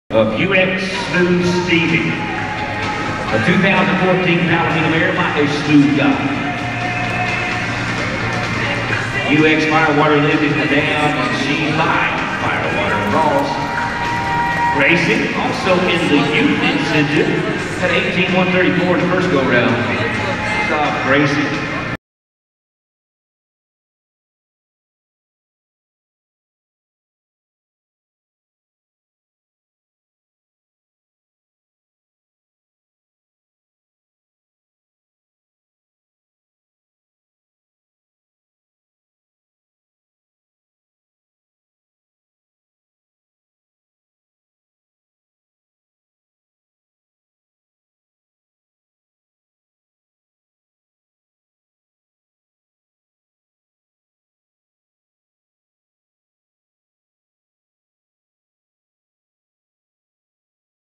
Gracie & Stevi: PINK BUCKLE 2021 Run
Don't worry, we'll definitely be back ;)
Here's the ghost run of the winning time vs. Gracie's time at the Pink Buckle Barrel Race Jackpot this year. She's sooooo close. We know what needs worked on for next year!